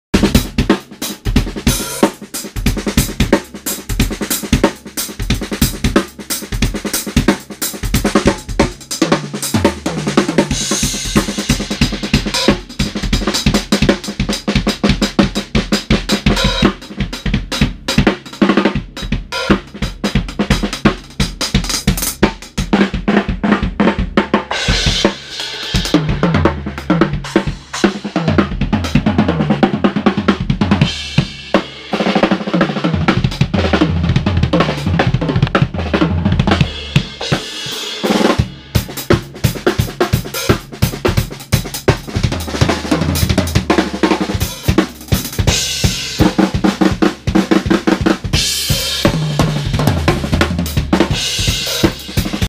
Jotan Afanador laying that shuffle with a twist
Jotan Afanador laying it down some shuffles with a twist and crazy fills (technique) in the studio! FOLLOW ON INSTAGRAM!
Canopus drums, Birch
Sabian Cymbals, 17 Legacy Crash, 18 Artisan Crash, 13 AA Dark proto type Hi Hats, 22 Big & Ugly Ride, 10' Mini Holy china stax on a 12' AA splash.
Vic Firth
Gon bops percussion
DW pedals
The Bass Drum Lift
KickPort
Rim Riser